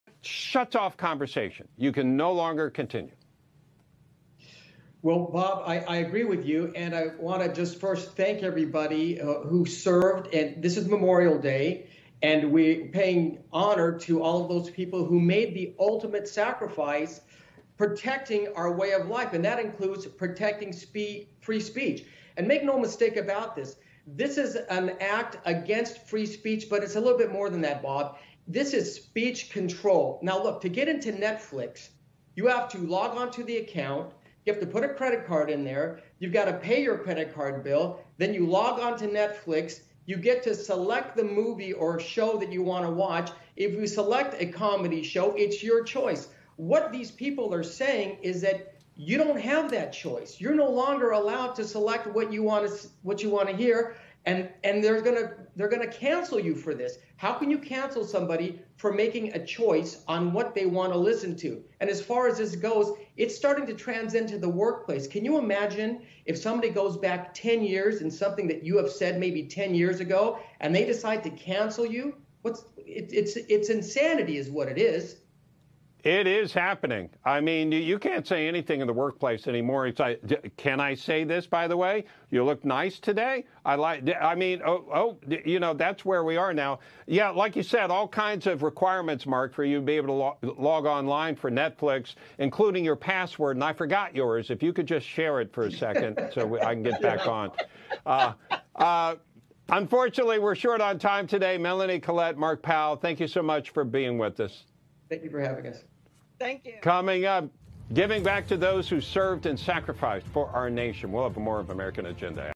Mark Powell NEWSMAX interview on why some want Netflix to remove the Ricky Gervais Comedy Show
Should Comedians be canceled for making jokes??? Isn’t that what comedians do??? Check out this 20 second news segment on the Ricky Gervais’s Netflix comedy show and find out what some are trying to do to stifle Free Speech…